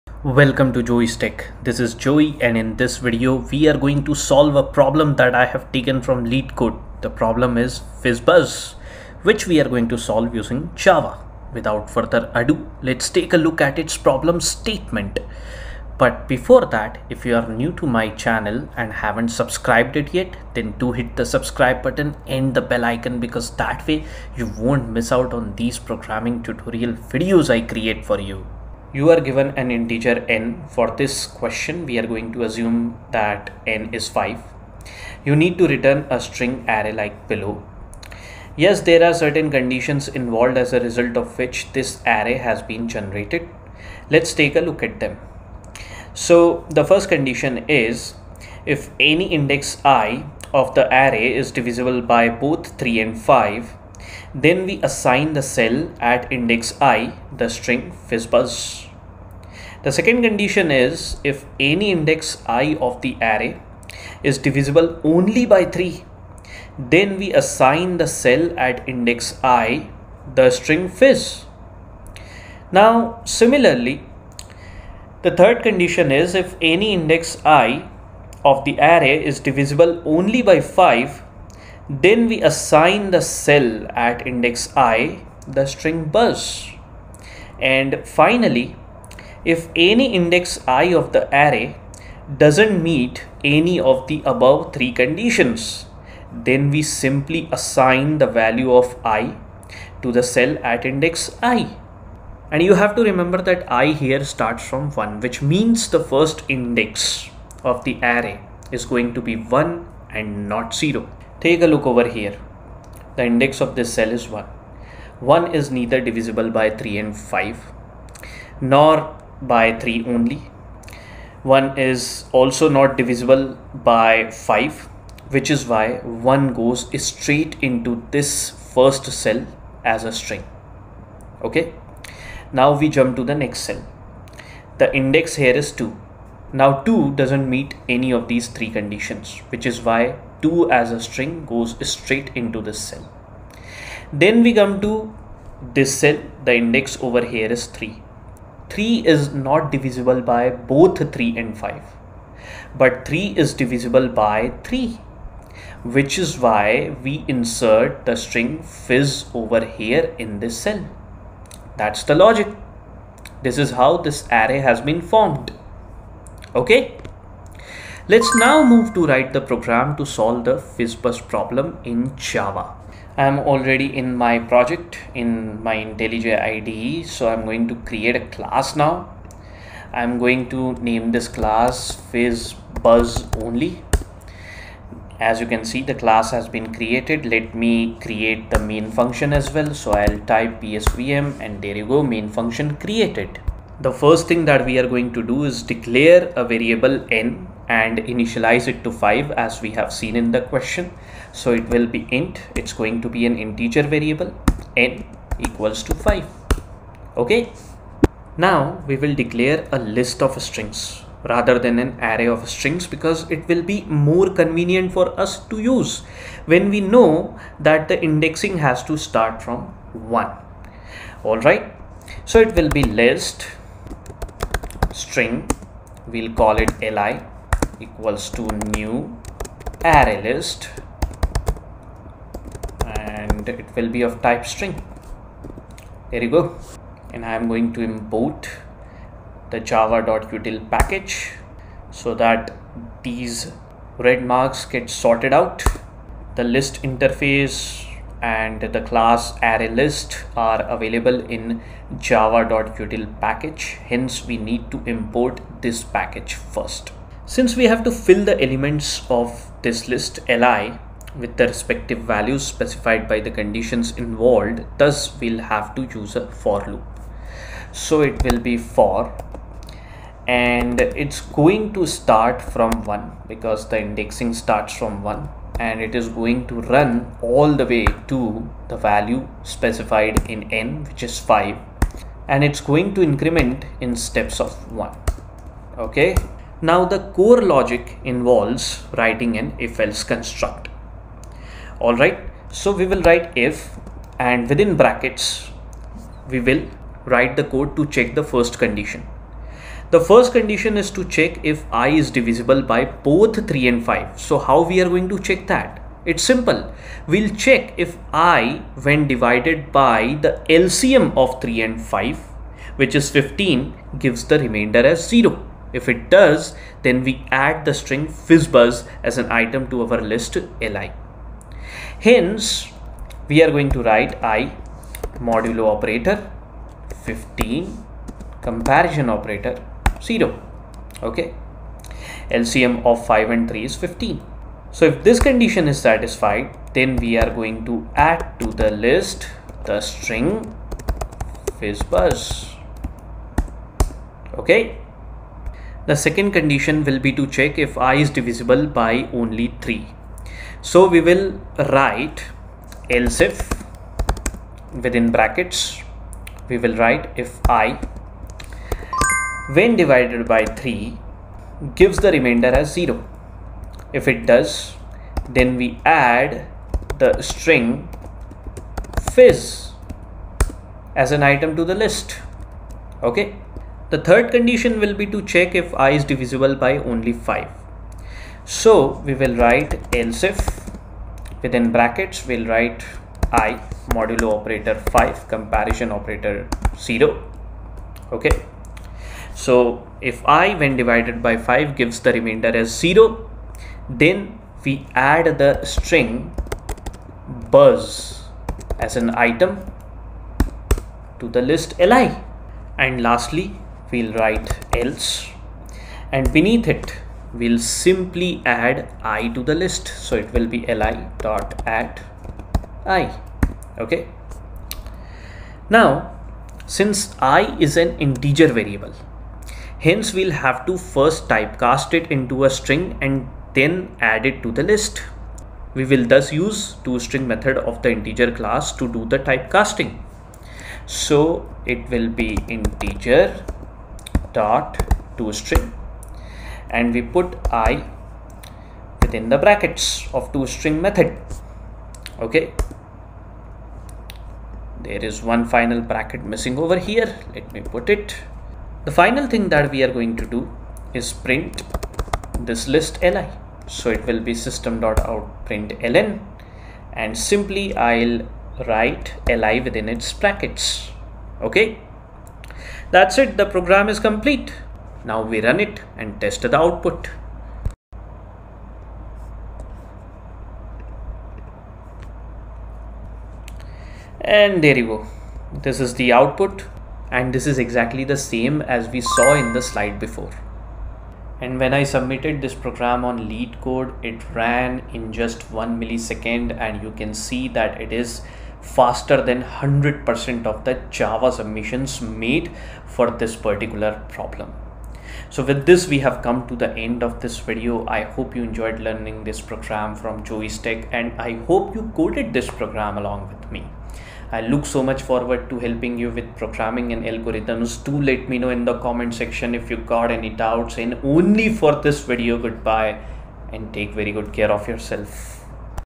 FizzBuzz | Leetcode 412 | Java solution explained step by step
Joey'sTECH brings you a complete step by step solution to the problem Leetcode 412 which is 'FizzBuzz'.

The program FizzBuzz in Java uses just one for loop to accomplish the task of finding to create the required string array. The Java program solves this problem of FizzBuzz Leetcode in linear time.

This is one more interesting addition to my array interview questions in Java

I have explained the entire algorithm to solve this problem and the java program step by step.

Also watch -